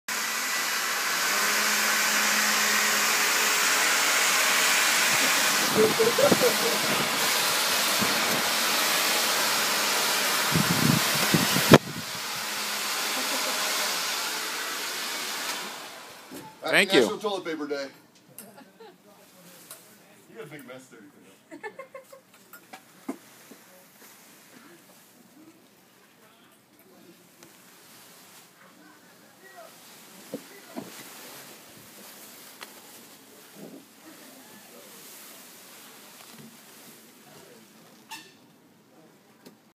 National Toilet Paper Day at P&G 2012
I have worked in a lot of offices over my 20 years at P&G, but this one has to be the nuttiest - 'Family Care East' houses Bounty, Puffs and Charmin.   Last week the General Manager was driving a motorized toilet through the halls.   Today, I'm told it is National Toilet Paper Day!!!